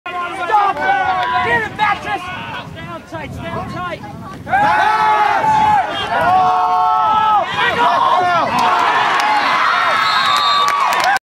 BFA Fairfax's Morosini gets badly needed 1st down
8 Sep 07 - Bullets Morosini catches 23 yard pass from Kirby for first down agaist CVU.